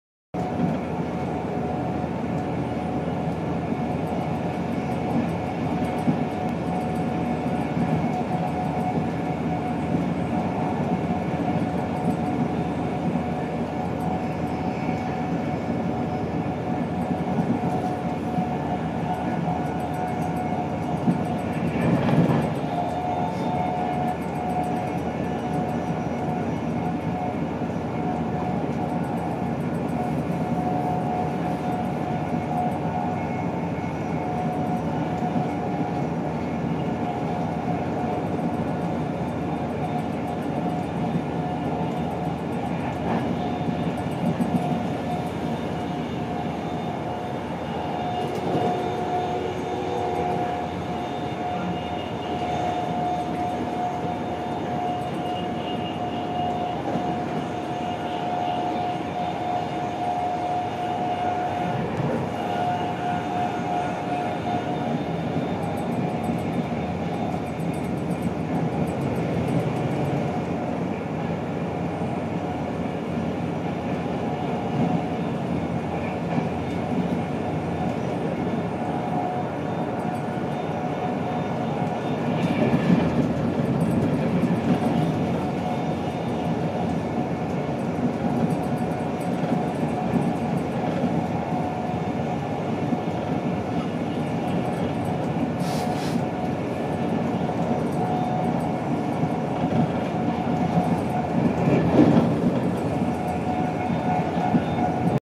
ECML on an LNER 225 (25/5/21)
Heading south through Nottinghamshire on a London North Eastern Railway 225.
Class 91 91111 'For the Fallen' is propelling, with DVT 82213 leading.
The 18:15 Leeds to London King's Cross service.

25th May 2021.